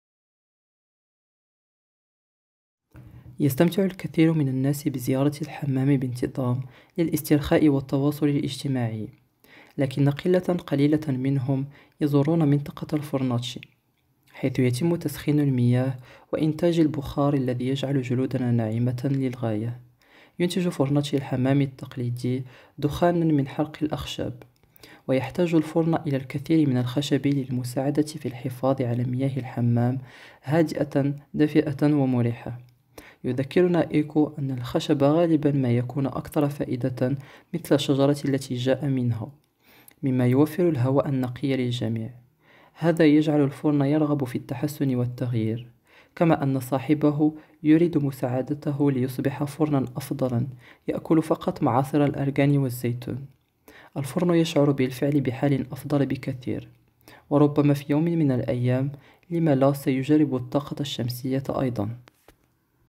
الحمام البيئي والطاقة
فيديو رسوم متحركة قصير عن الحمامات البيئية والطاقة

ترجمة معاد محطوش. التعليق الصوتي لأيمن درموش